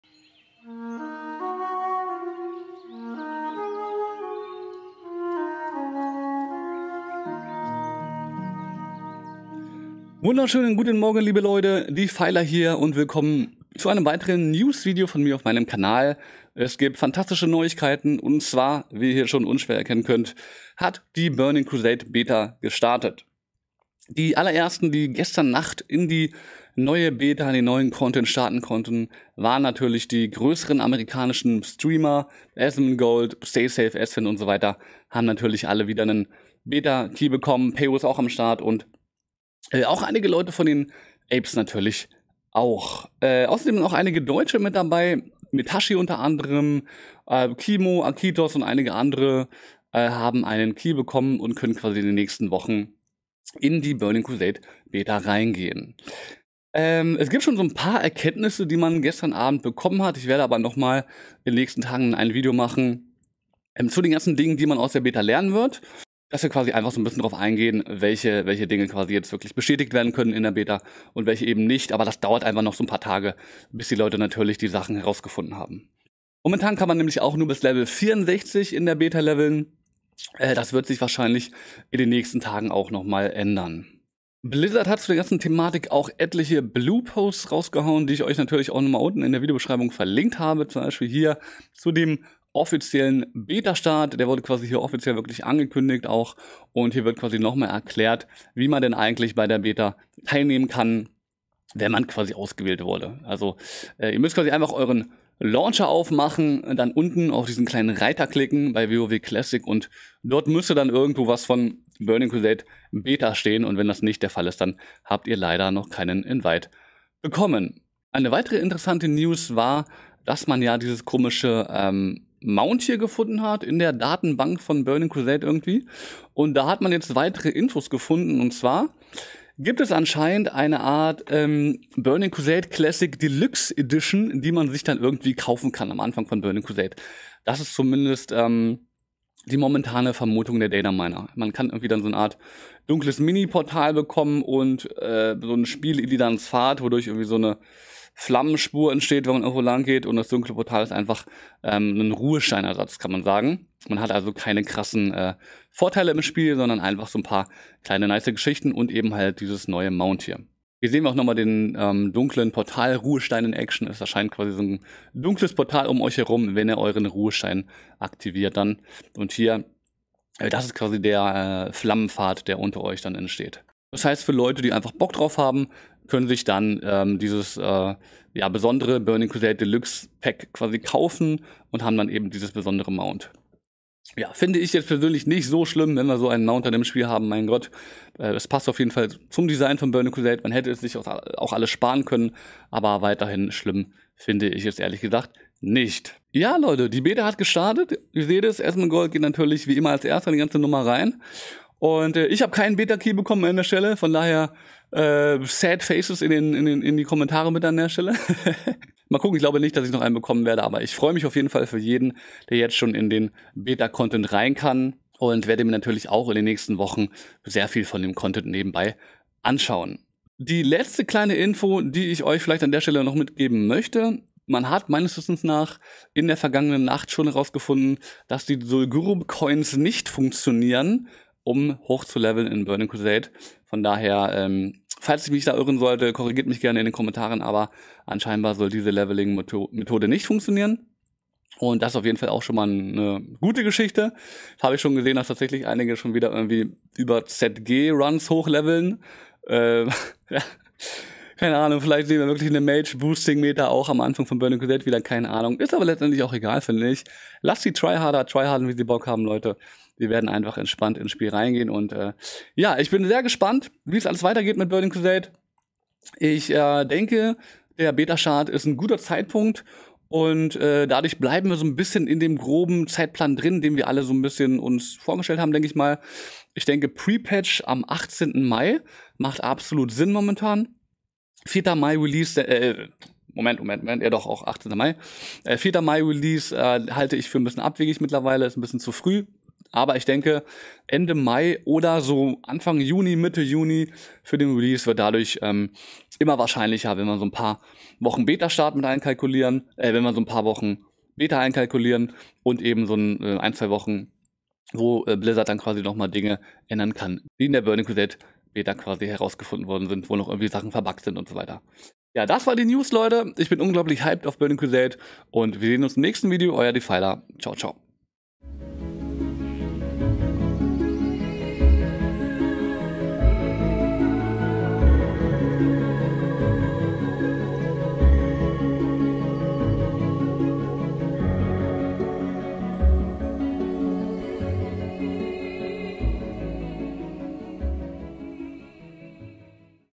DIE TBC BETA BEGINNT! 😱
(Sonntag, Mittwoch & Freitag von 15 - 20 Uhr live!)

ENDLICH! Die TBC Classic Beta hat begonnen! In der Nacht von Dienstag auf Mittwoch konnten die ersten großen US Streamer in der Beta durch das dunkle Portal in die Outlands treten!
Asmongold, Esfand und Co. sind natürlich wieder mit einem Beta-Key ausgestattet! Zudem sind auch einige Deutsche dabei: Metashi, Kimuh, Akitos und einige andere dürfen sich über einen Zugang freuen!
Ich bin gespannt auf die nächsten Wochen mit The Burning Crusade Classic Content und denke ein Release Anfang/Mitte Juni wird immer wahrscheinlicher!
LG Defiler